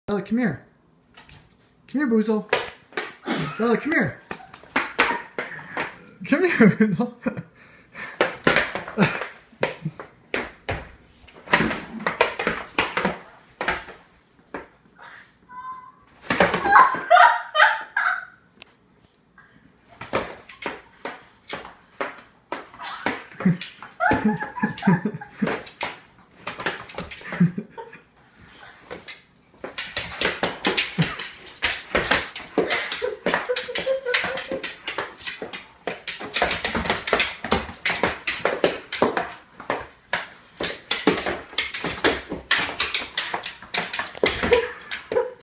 Bella chasing the laser with doggy shoes on
Our funny boxer dog loves chasing the laser. This time we put doggy shoes on. Dog Vs. Hard wood floors.